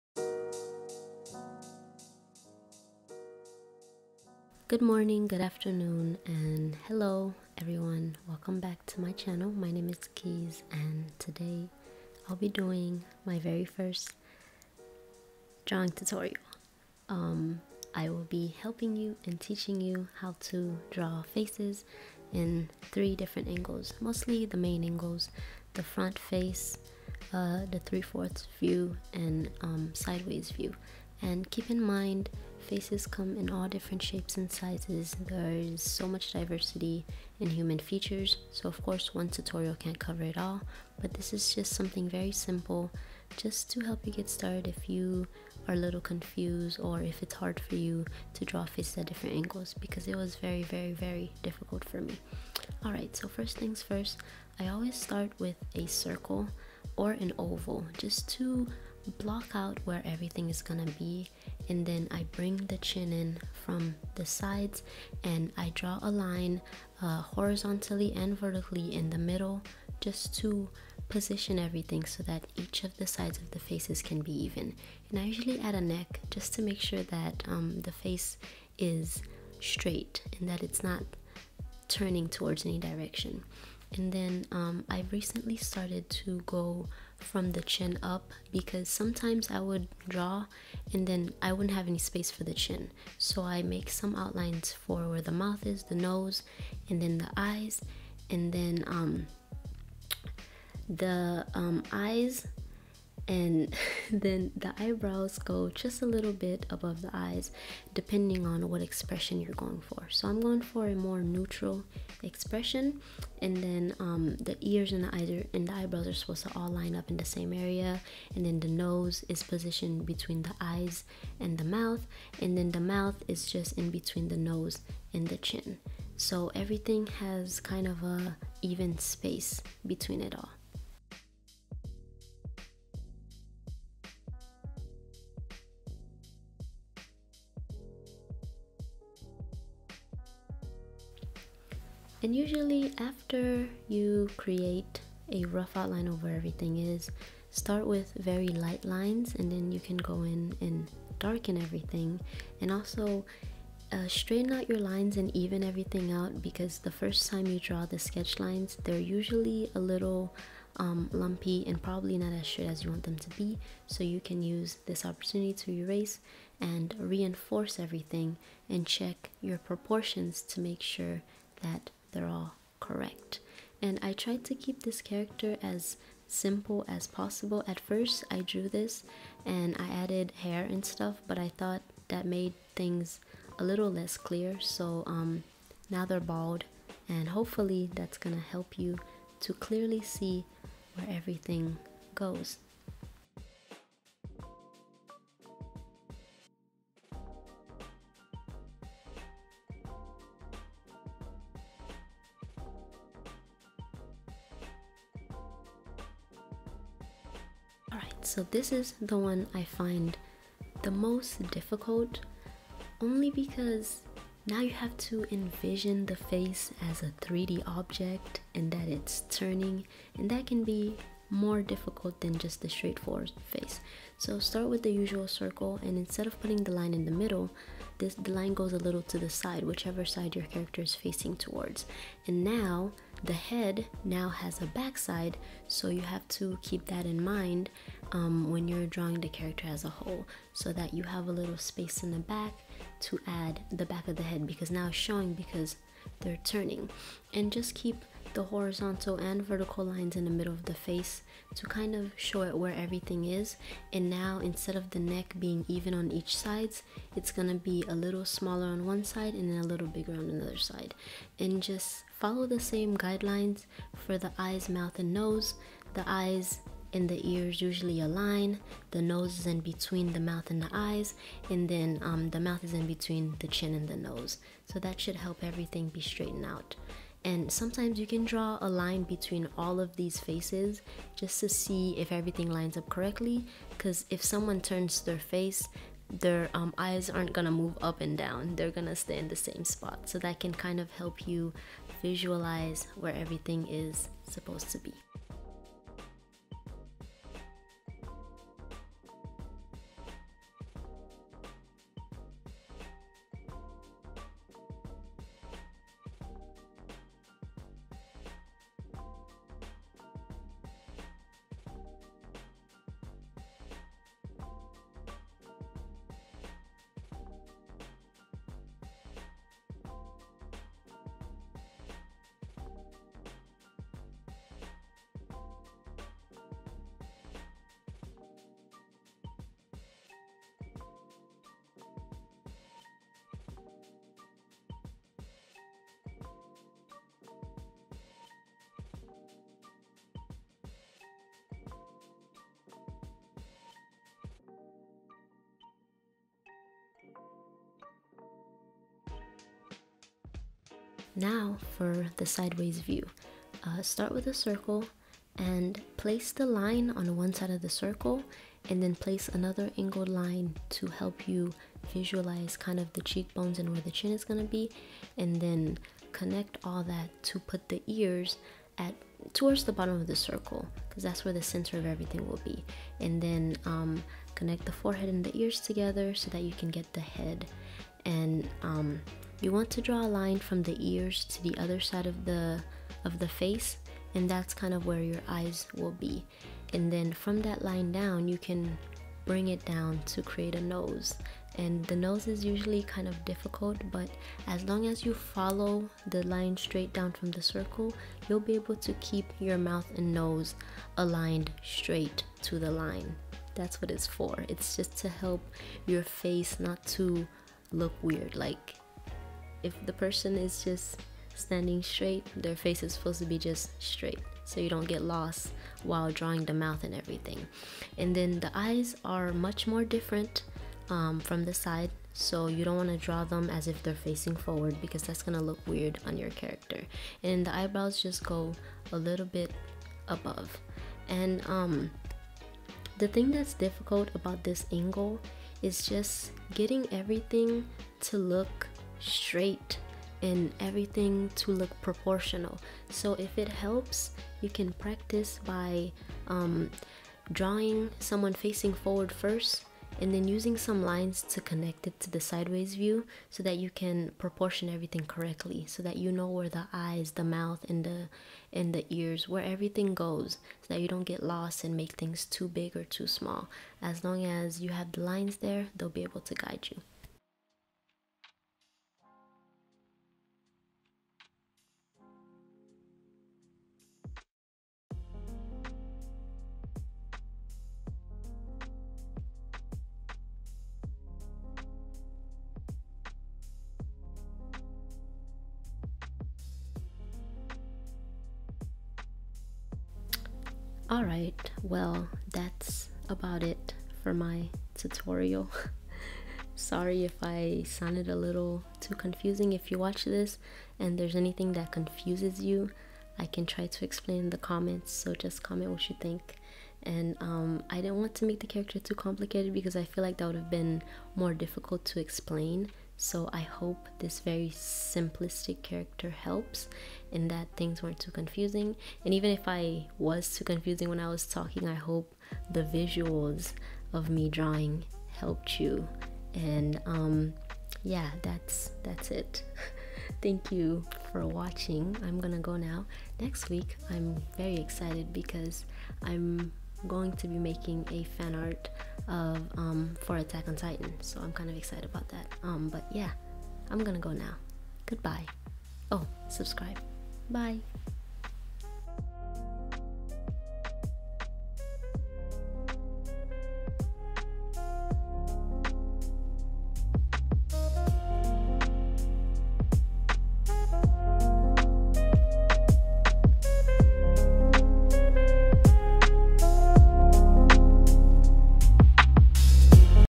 How to draw faces (~˘▾˘)~
How to draw faces ,thank u for watching this drawing tutorial! I hope that it helps
I've changed my insta from Kise_art to Kiseartt.I've also begun posting more art!

Bye-Bye-